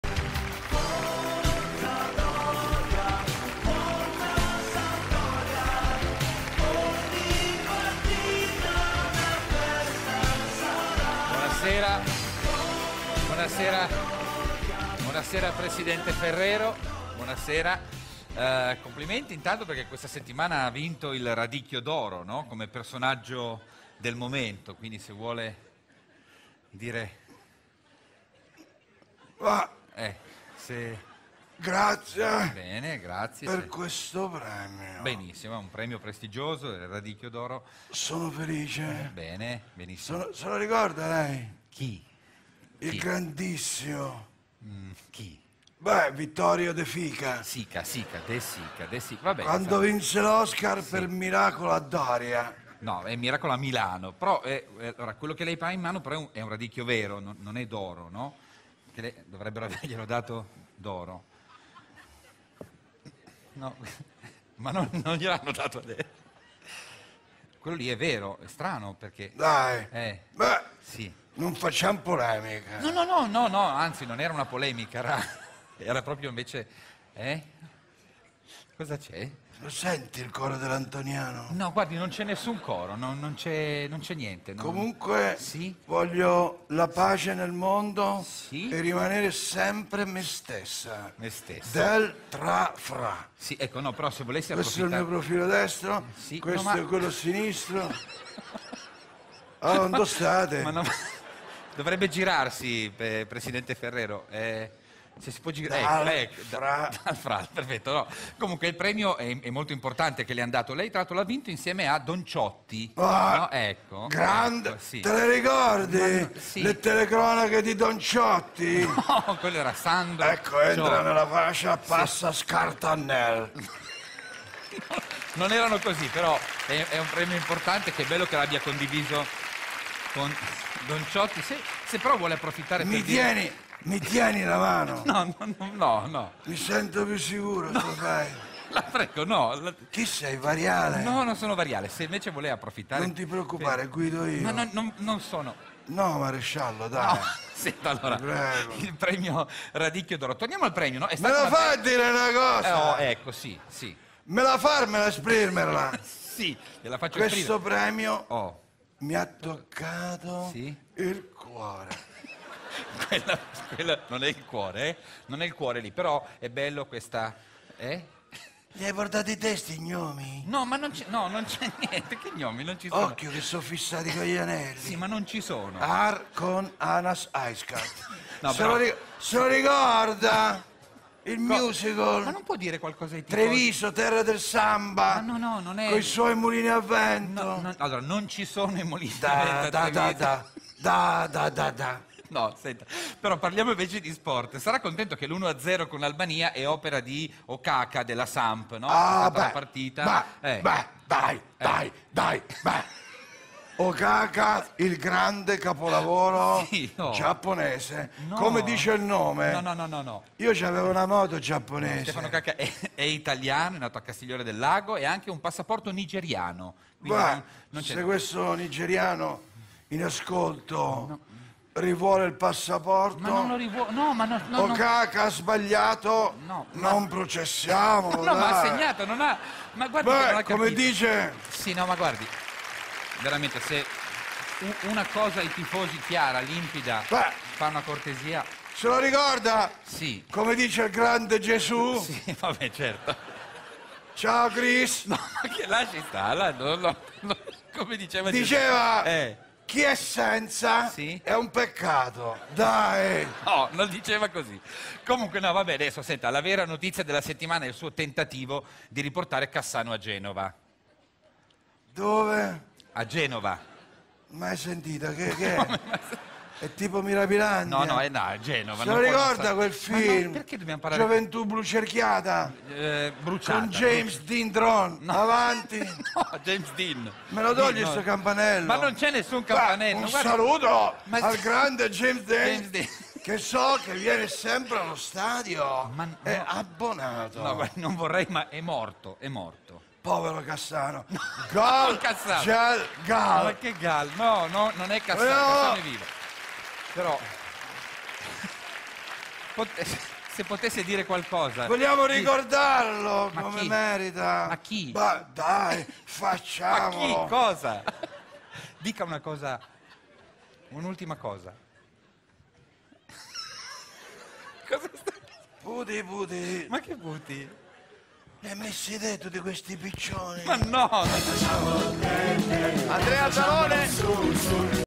Crozza-Ferrero vincitore del premio Radicchio d’oro 2014
'La posso dirla una cosa?.. Me la farmela esprimelala? Questo premio mi ha toccato il cuore..'